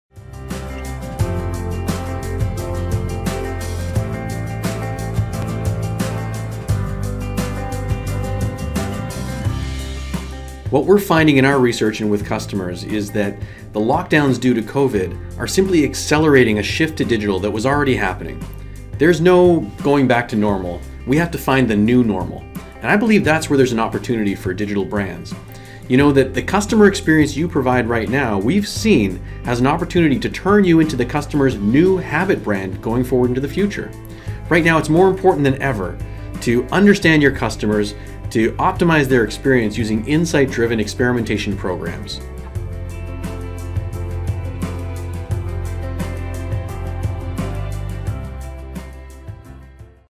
Thriving after the crisis - How brands can build a digital strategy for future growth
24% of consumers will increase long-term use of digital channels due to the Covid-19 crisis : How brands should respond to consumers expectations?